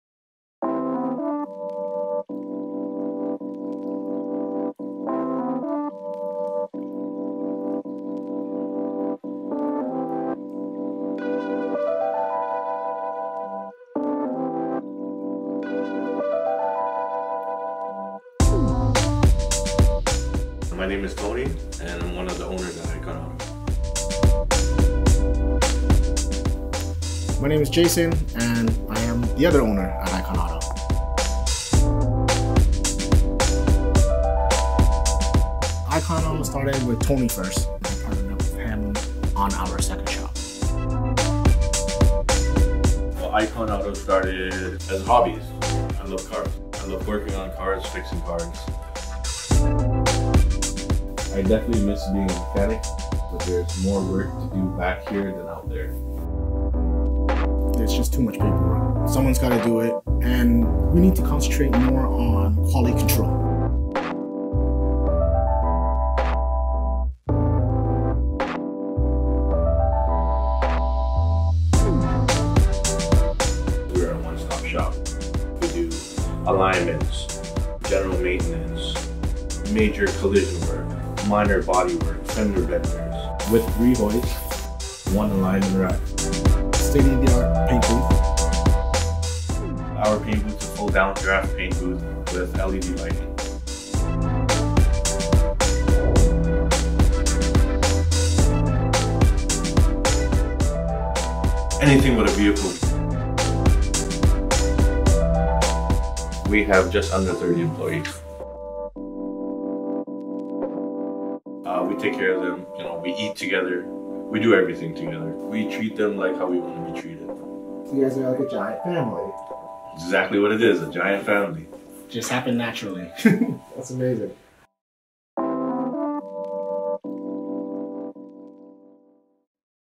Icon Auto (Auto Shop Commercial Example)
Jumping Elephant had the pleasure of working with Icon Auto, an all-in-on auto shop based in Etobicoke, ON, Canada. (before rebranding to Jumping Elephant Productions)

They are a professional, passionate, and trustworthy business that provides top of the line auto bodywork. Their dedication to strive for the best service is very commendable.

Icon Auto has a large facility with around 30 employees to service your vehicle. They service small to big body works. They work with common to exotic vehicles (they have a Master Mechanic on-site). Finally, they take pride in their state of the art paint booth that has professional painters, equipment, and artwork.

Visit them today at
19 Advance Rd, Etobicoke, ON M8Z 2S6

Film Crew:
Editor/Camera 1: Jerome Esguerra
Producer/Interviewer/Camera 2: Sang Nguyen